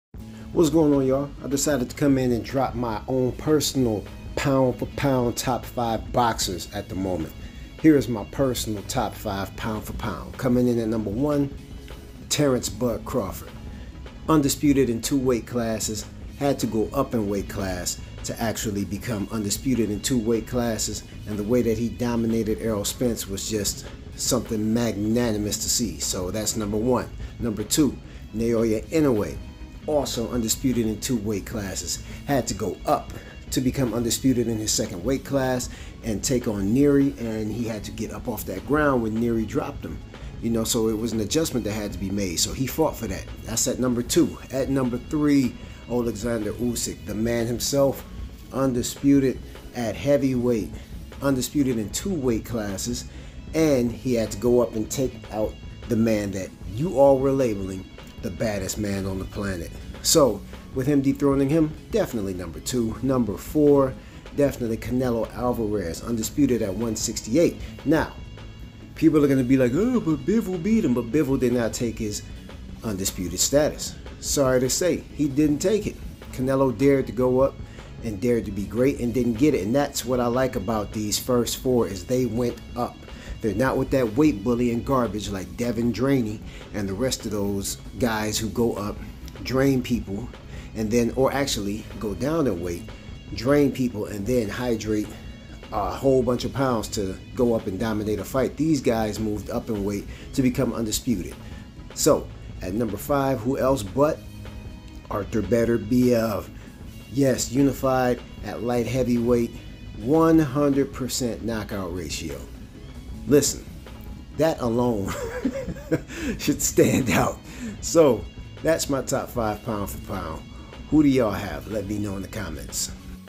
My current Pound for Pound top 5 rankings
This is my current list of boxings top 5 Pound for Pound rankings.

Agree or disagree? If you disagree, who are you current top 5?